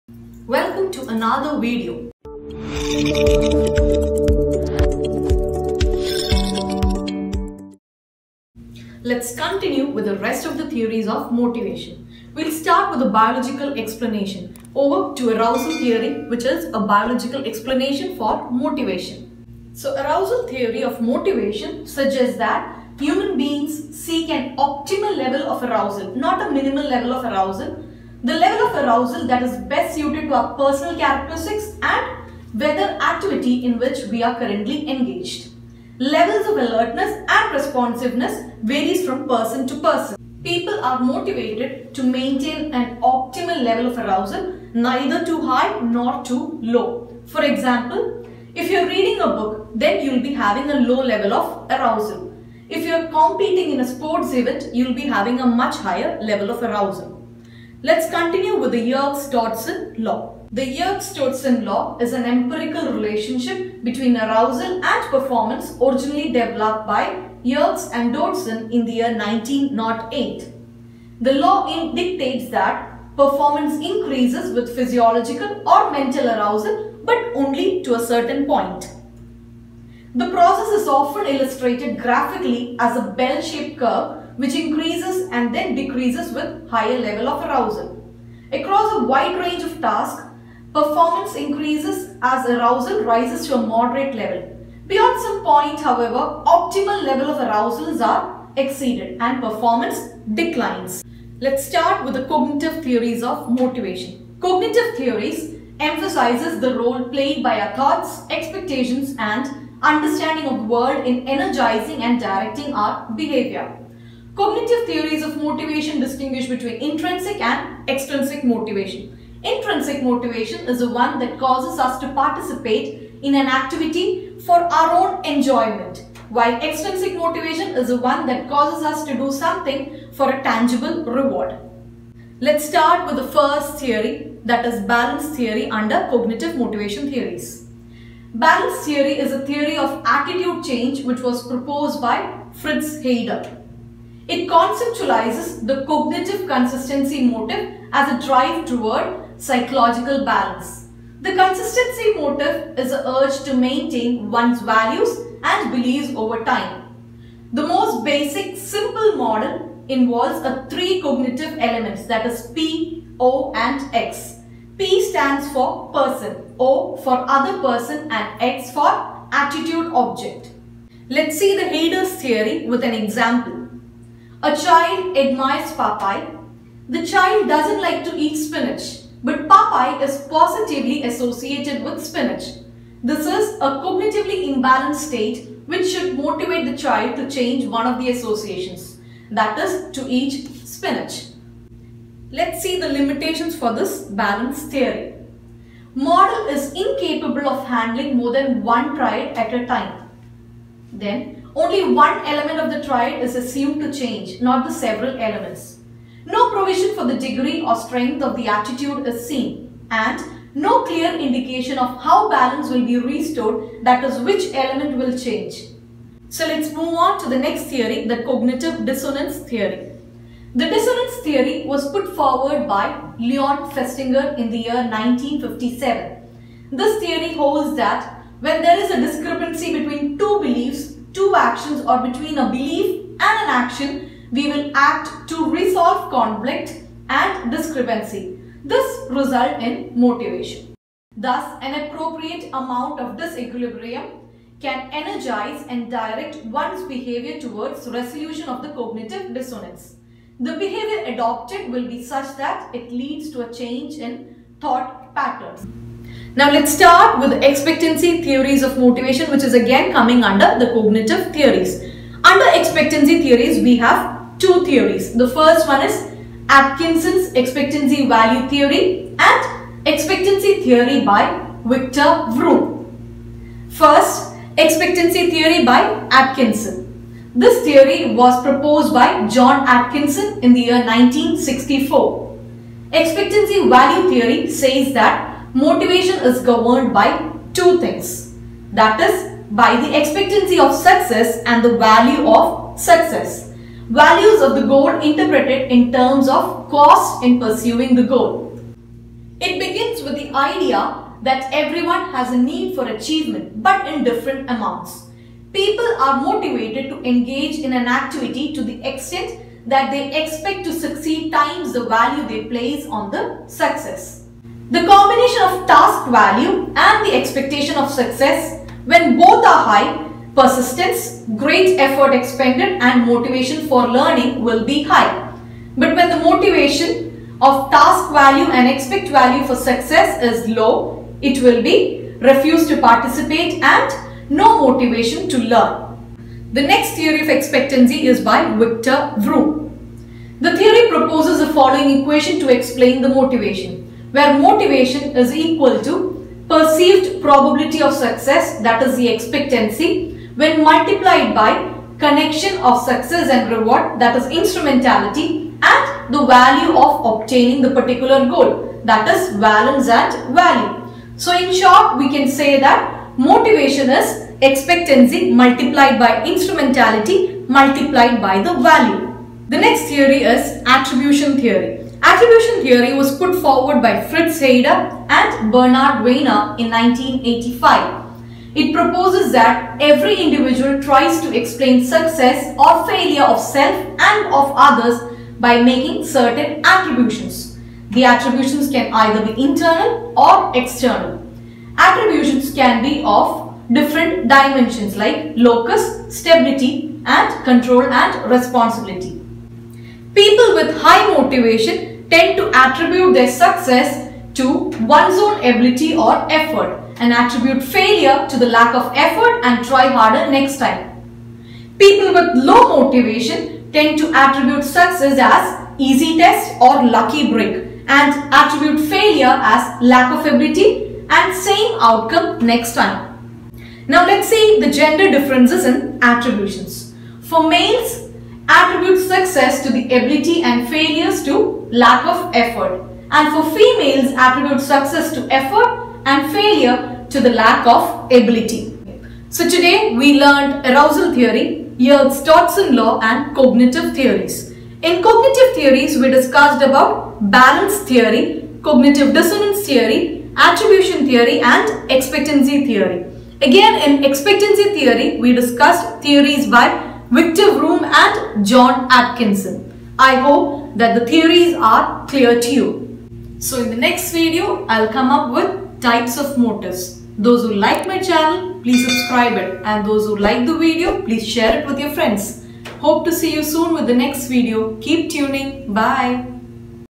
Theories of Motivation | General Psychology I KALOPSIA
Arousal Theory
Yerkes-Dodson Law
Cognitive Theories of Motivation
Balance Theory
Cognitive Dissonance Theory
Attribution Theory
Expectancy Theory
Atkinson value expectancy Theory
Expectancy Theory by Victor Vroom

Theories of Motivation part 01👇🏻